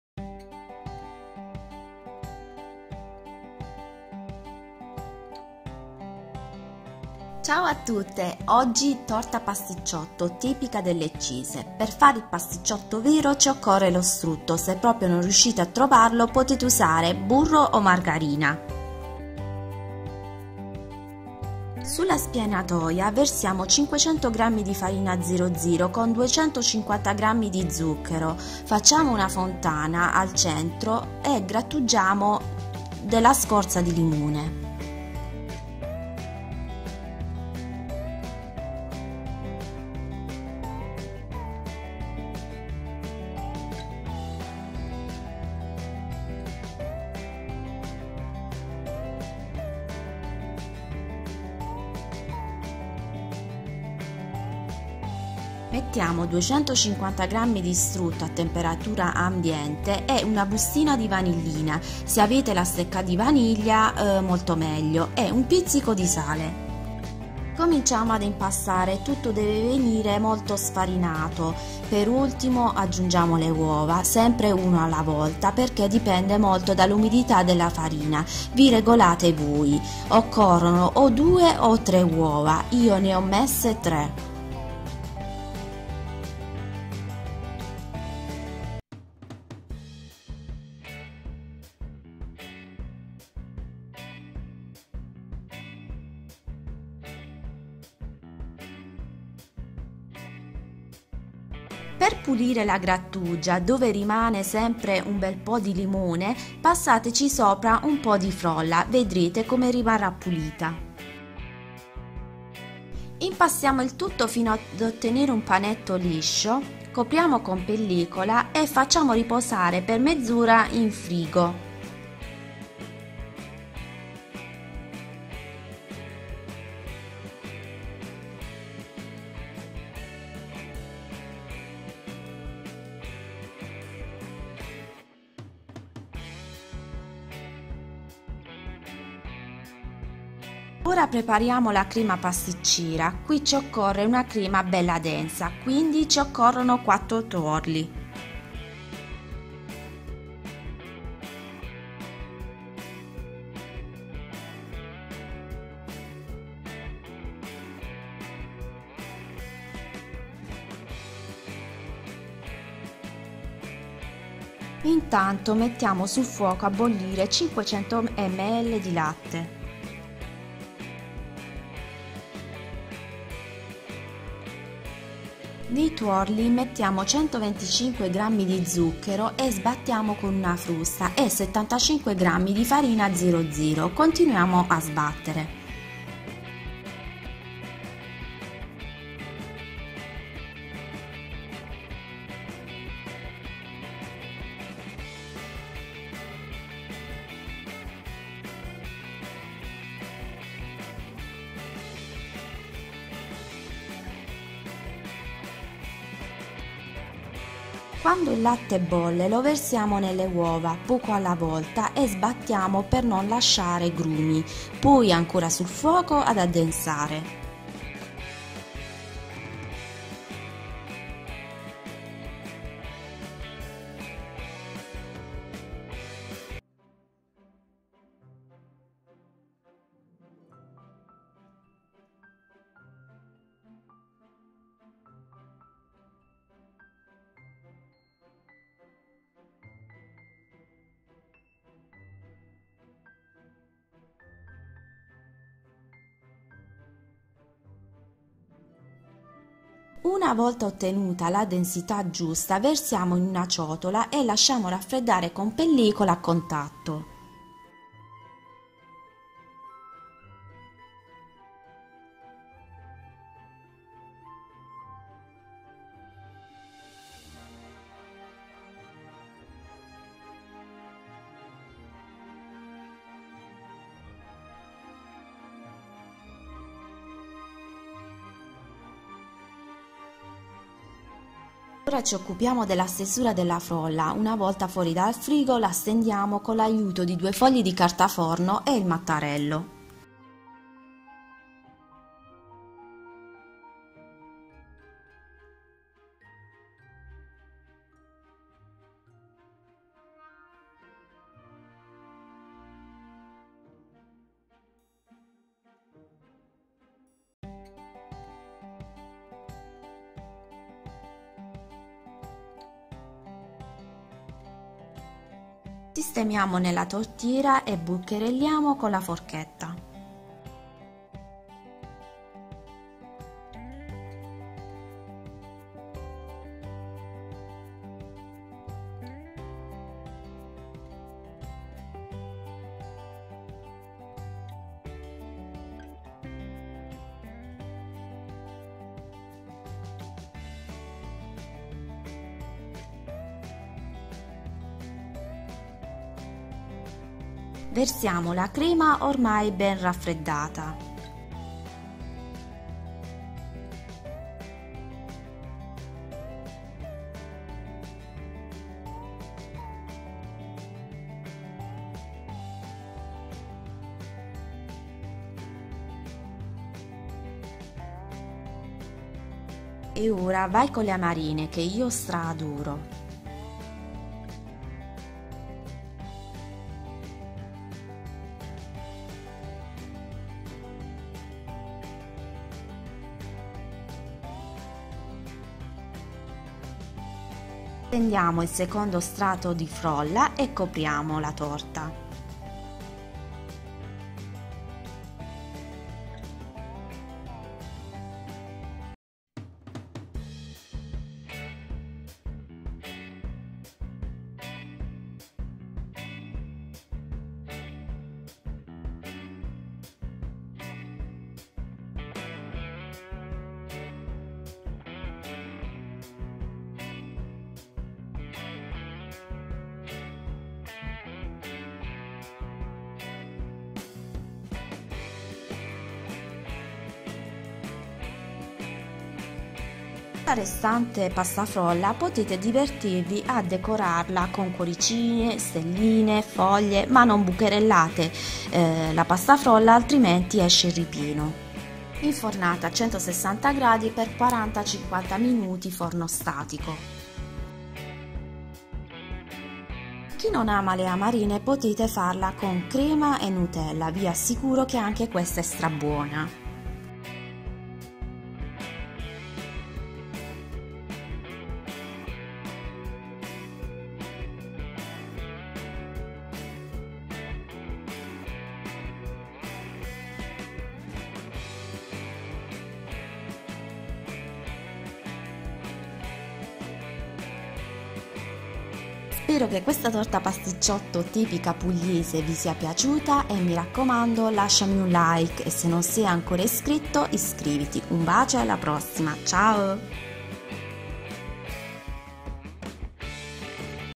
Torta pasticciotto leccese,crema e amarene
I pasticciotti dolci tipici pugliesi,da assaggiare assolutamente chi viene in Puglia,pasta frolla friabilissima ripiena di crema e amarene oppure crema e nutella.

INGREDIENTI per il vero pasticciotto tipico leccese:
500 gr di farina 00
250 gr di strutto
250 g di zucchero
2 o 3 uova
scorza di limone
 vanillina